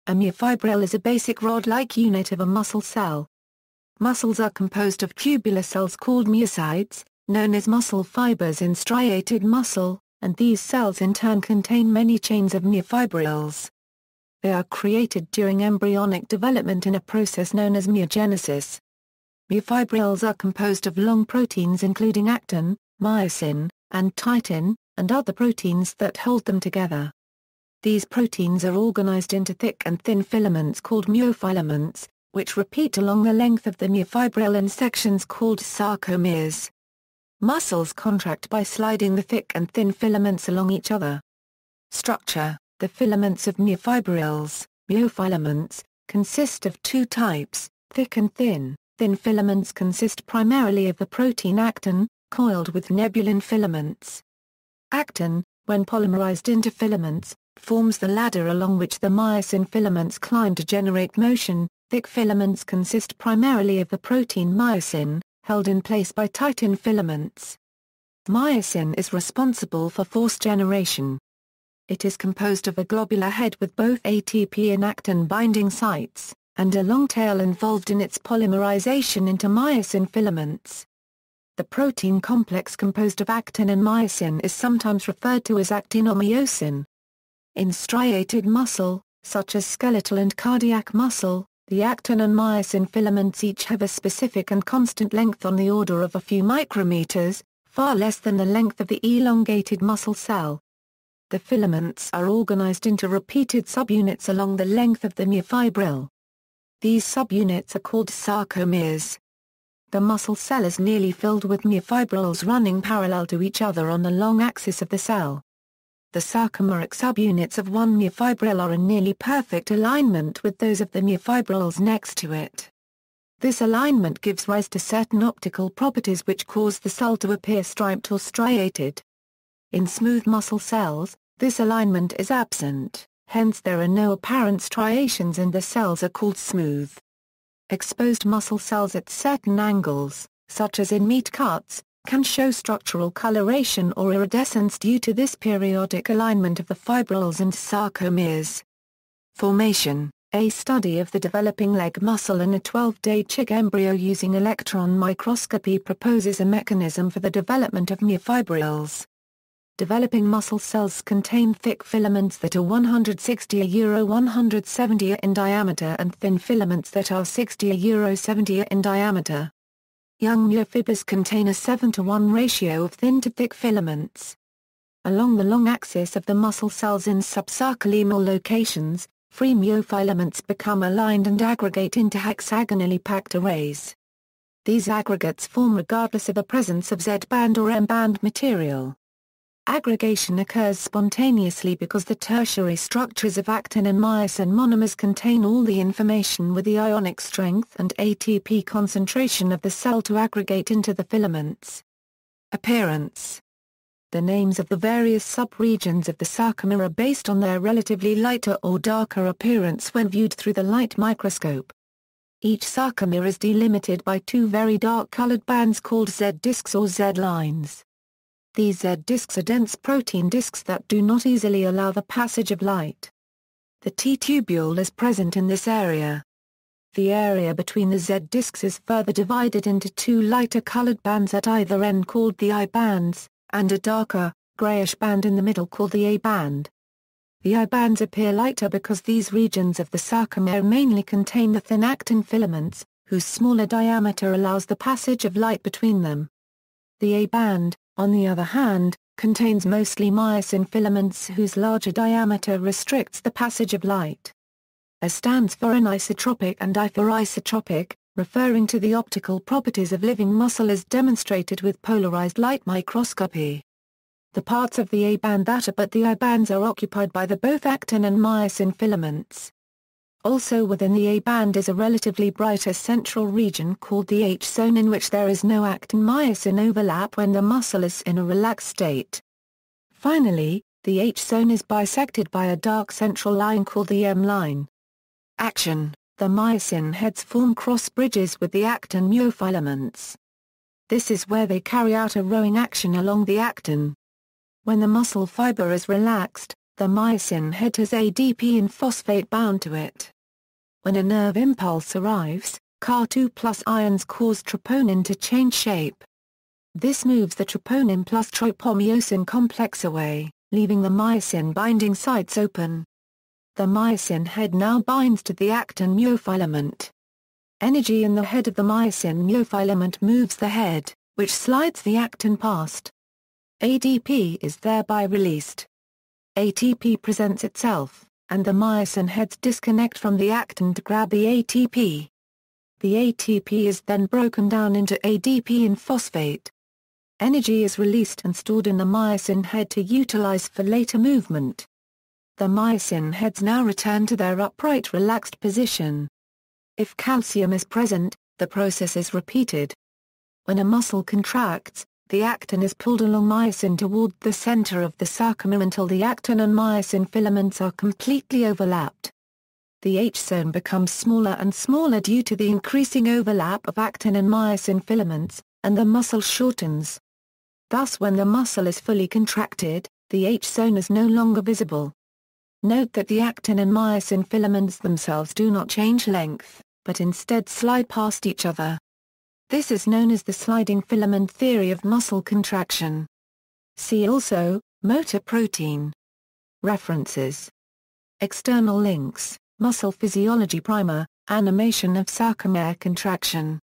Myofibril
A myofibril is a basic rod-like unit of a muscle cell. Muscles are composed of tubular cells called myocytes, known as muscle fibers in striated muscle, and these cells in turn contain many chains of myofibrils. They are created during embryonic development in a process known as myogenesis.
Myofibrils are composed of long proteins including actin, myosin, and titin, and other proteins that hold them together. These proteins are organized into thick and thin filaments called myofilaments, which repeat along the length of the myofibril in sections called sarcomeres. Muscles contract by sliding the thick and thin filaments along each other.